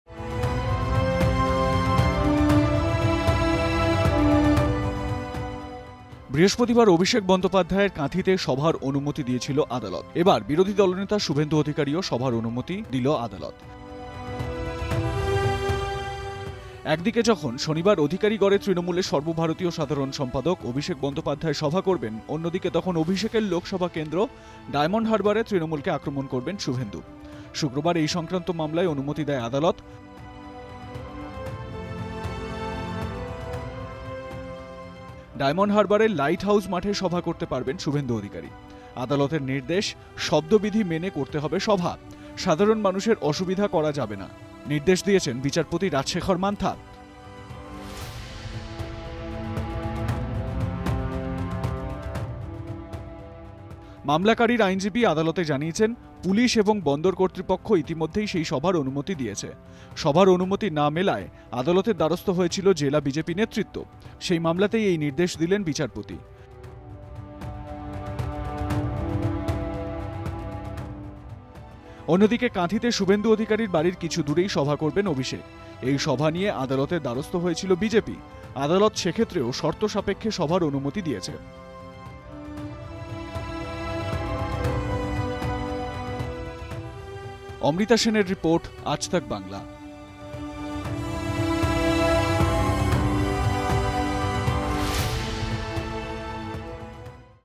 শুভেন্দুর সভার অনুমতি মিলল | Suvendu Adhikari | BJP | Aaj Tak Bangla
বৃহস্পতিবার অভিষেক বন্দ্যোপাধ্যায়ের কাথিতে সভার অনুমতি দিয়েছিল আদালত। এবার বিরোধী দলনেতা শুভেন্দু অধিকারীও সভার অনুমতি দিল আদালত। একদিকে যখন শনিবার অধিকারী গড়ে তৃণমূলের সর্বভারতীয় সাধারণ সম্পাদক অভিষেক বন্দ্যোপাধ্যায় সভা করবেন। অন্যদিকে তখন অভিষেকের লোকসভা কেন্দ্র ডায়মন্ড হারবারে তৃণমূলকে আক্রমণ করবেন শুভেন্দু।